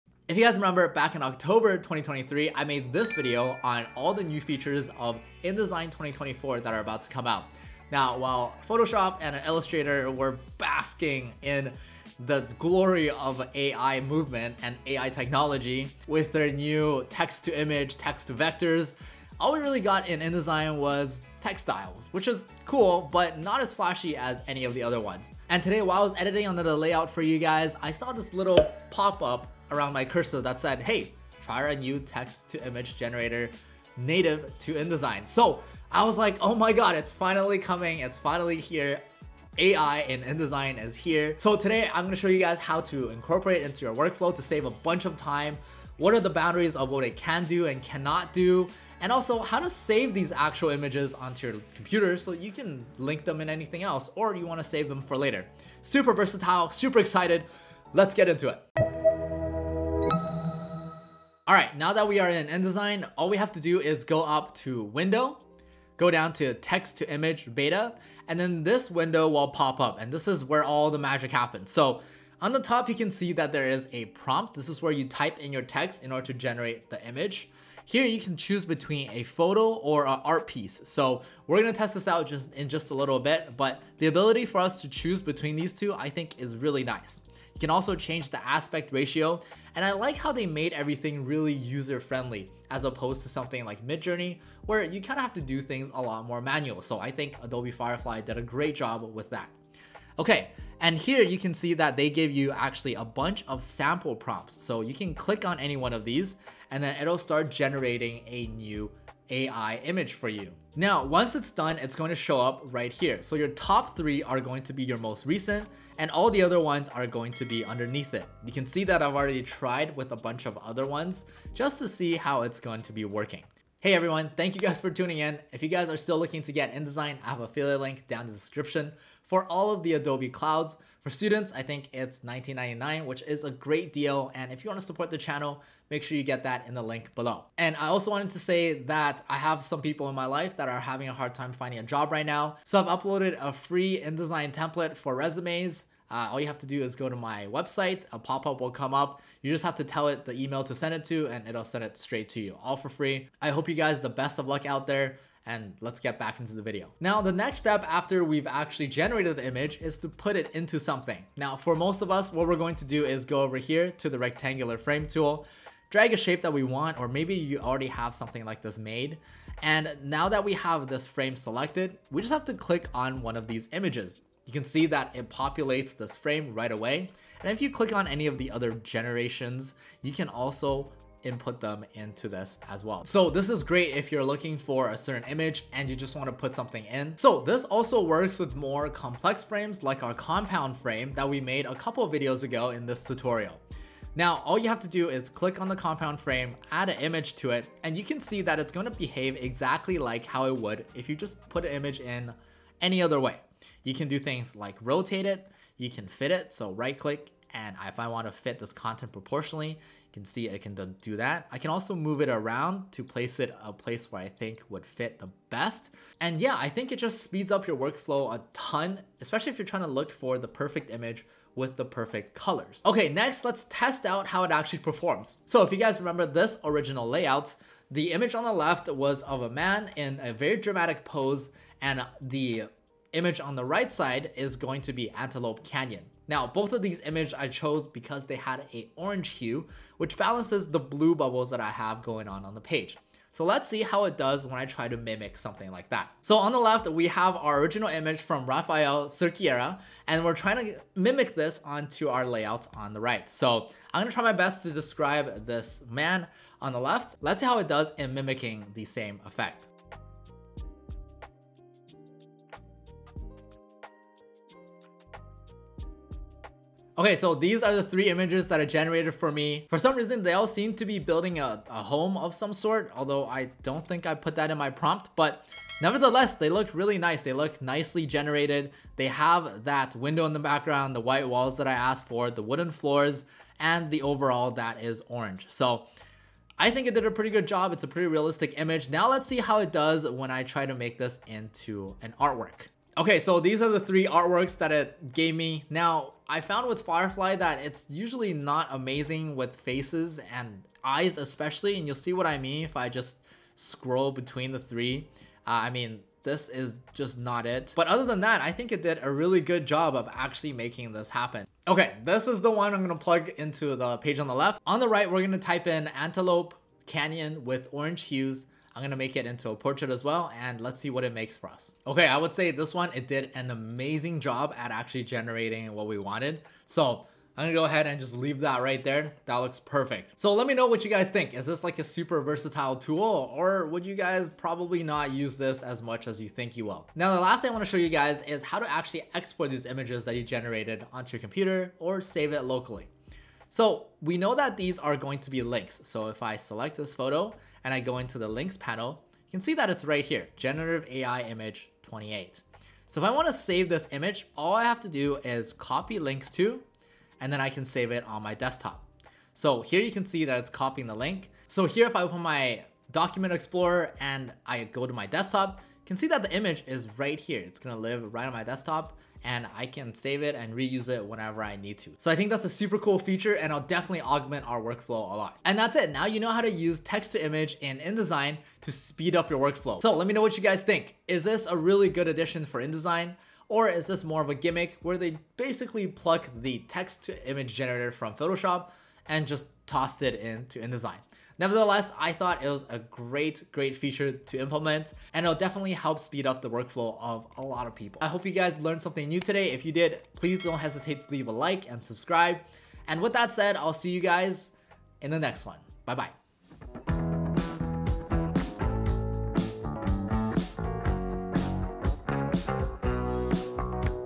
GENERATIVE AI in InDesign! | Speed up Workflow 5x
Generative AI is finally in InDesign! Today we take this chance to see what the changes from April 2024 has brought to InDesign and how we can utilize the new tools that are presented to us. In this video we will go over how to use generative Ai, how it might supplement our workflow, what are the limitations of the feature, and how to save generated images onto your computer.